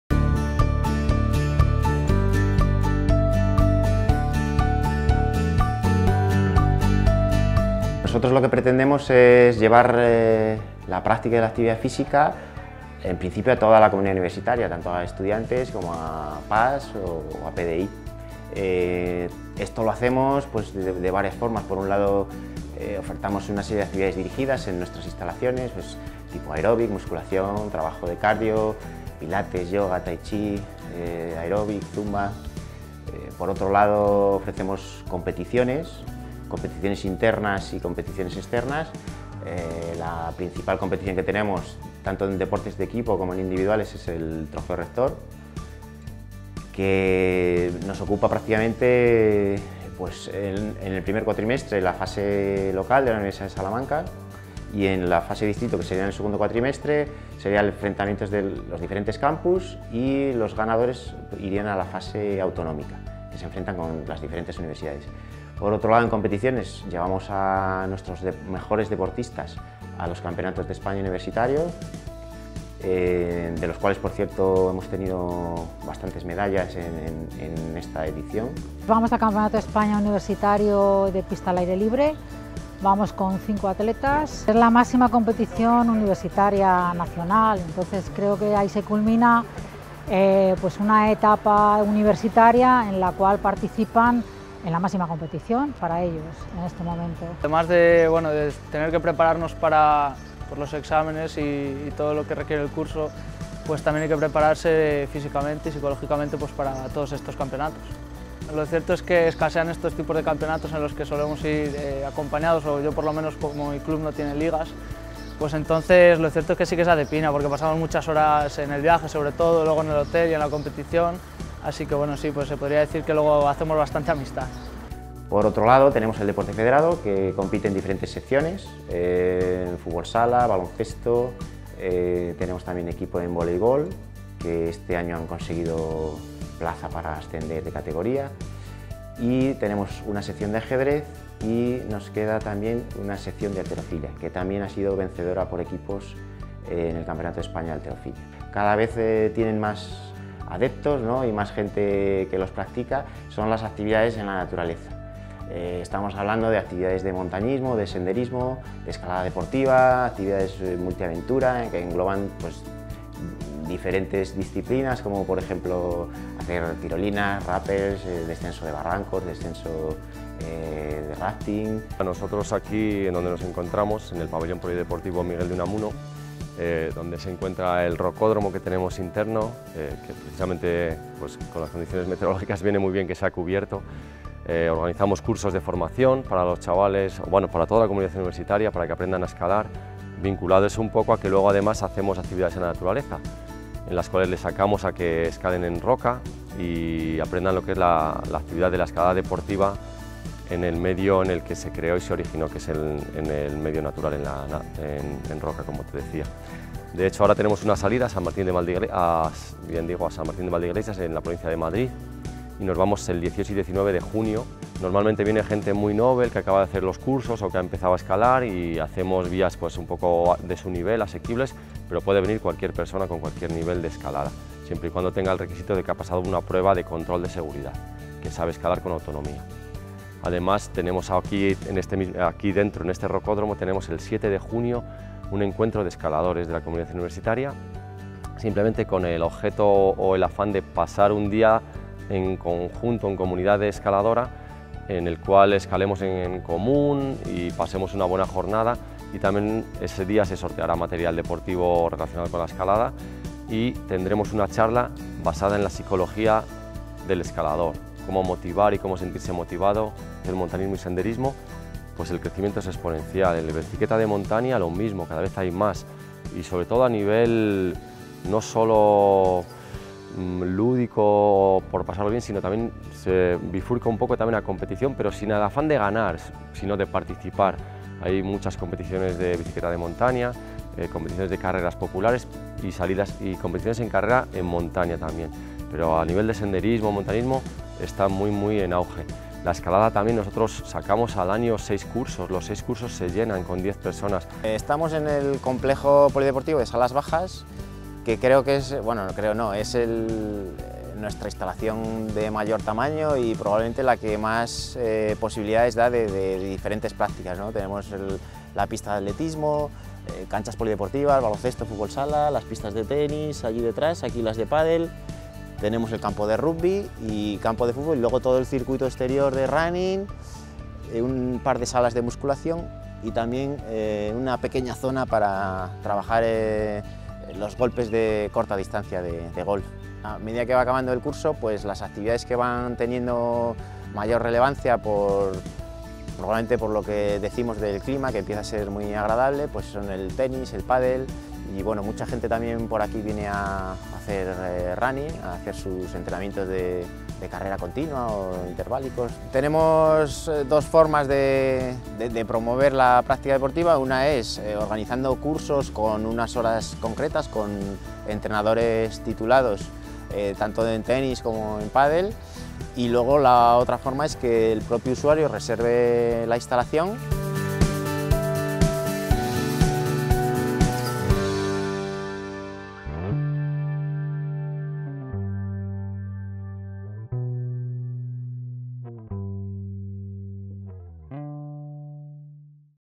Primavera Deportiva
Te descubrimos todas las actividades que ofrece a la comunidad universitaria el Servicio de Educación Física y Deportes de la Universidad de Salamanca